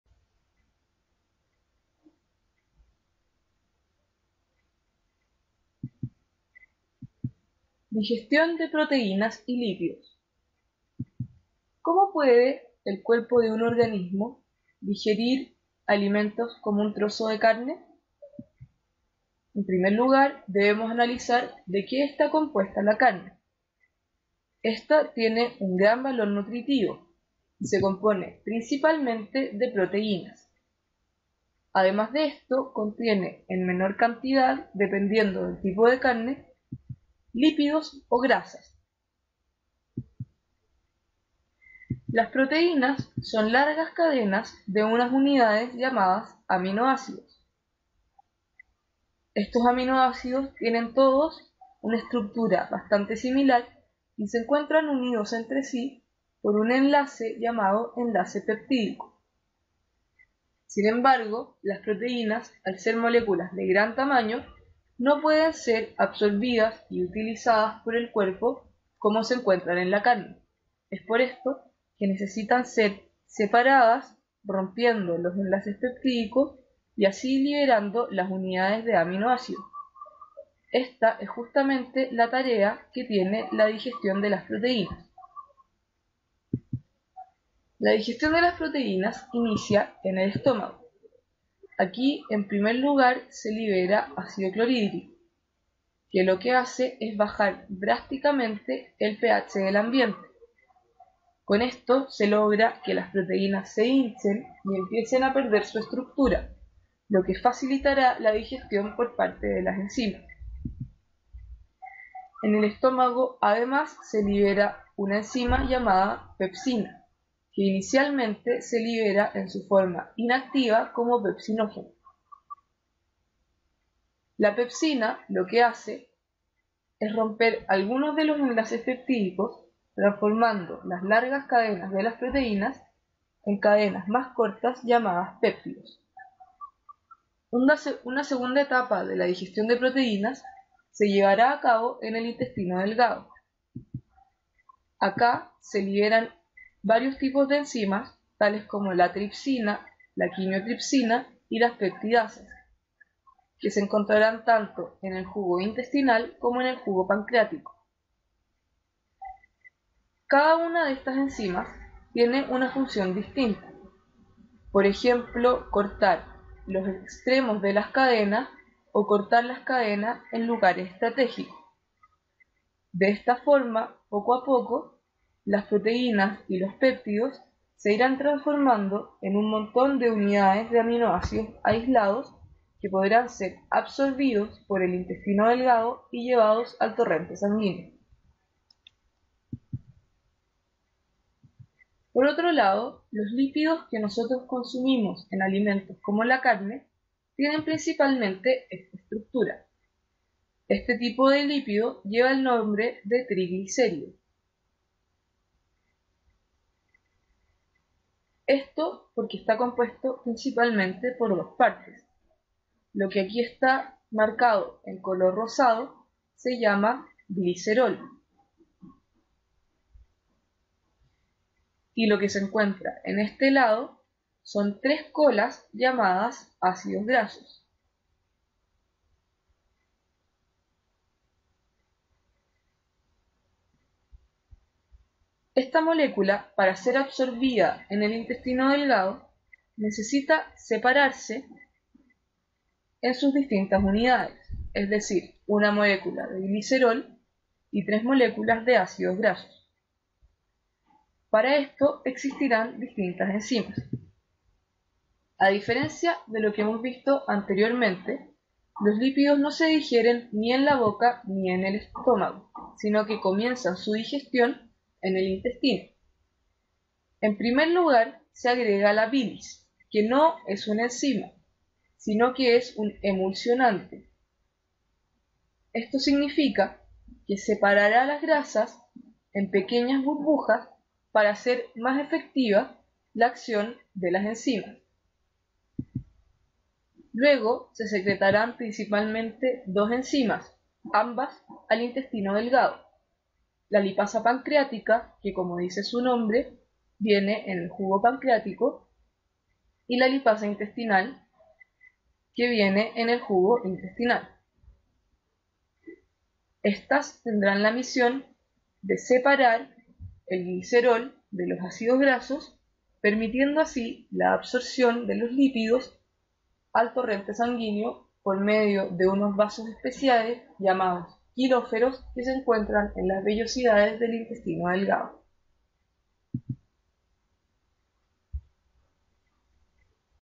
Digestión Proteínas y Lípidos
Digestión de los componentes de un pedazo de carne